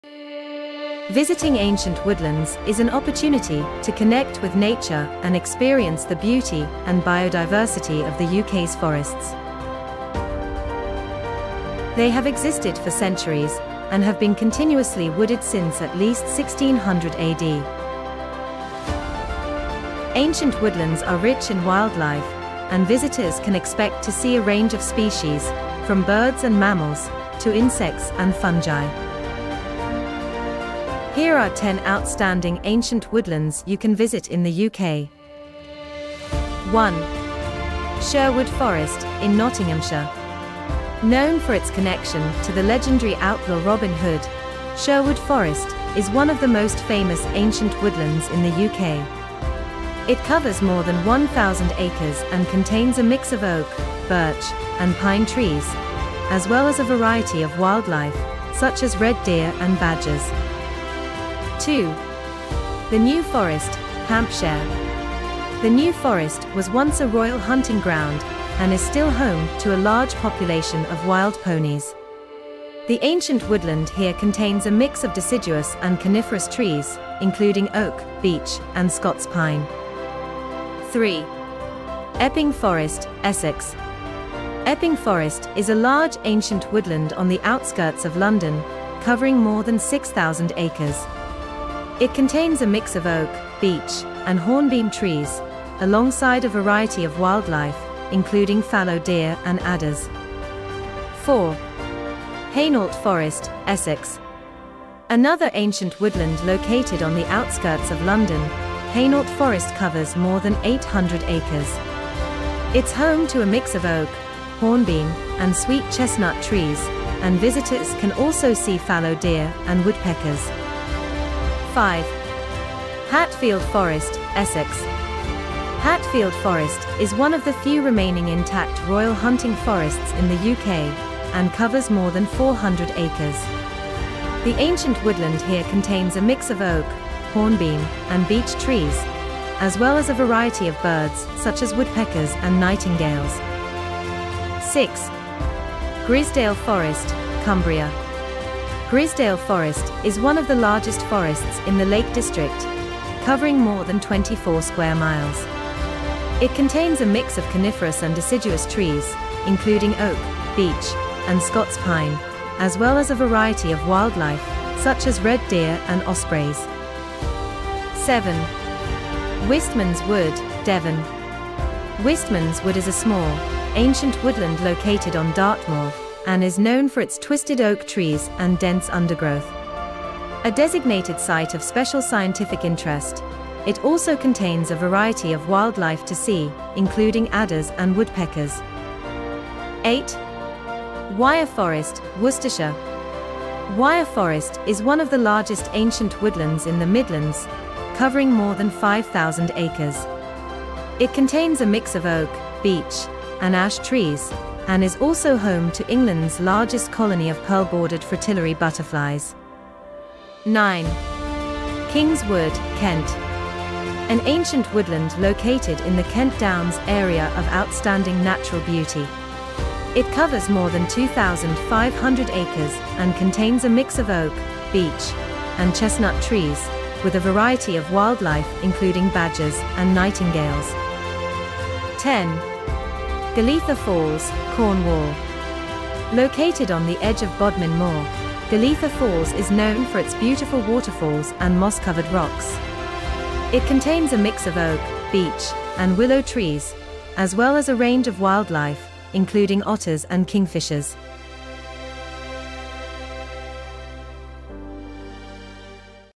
Top 10 MUST VISIT Ancient Woodlands in the UK
Enter the UK's ancientwoodlands and immerse yourself in the splendour of these age-old ecosystems for an unusual but special adventure. From the enchanting beauty of the New Forest to the mysterious charm of Sherwood Forest, these woodlands harbour a wealth of biodiversity, history, and tranquility. Roam amidst towering trees, listen to the symphony of nature's melodies, and encounter rare flora and fauna that have flourished for centuries. Whether it's the captivating bluebells in spring or the golden hues of autumn, each season unveils a unique spectacle. Discover hidden pathways, uncover historical landmarks, and witness the magic of these woodlands, where time seems to stand still. Visiting ancient woodlands offers not only a rejuvenating escape but also a profound connection to the natural heritage that has shaped the UK's landscapes for generations.

0:00 Introduction
0:37 Sherwood Forest
1:02 The New Forest
1:21 Epping Forest
1:41 Hainault Forest
2:02 Hatfield Forest
2:25 Grizedale Forest
2:48 Wistman's Wood
3:10 Wyre Forest
3:31 King's Wood
3:52 Golitha Falls
4:12 Outro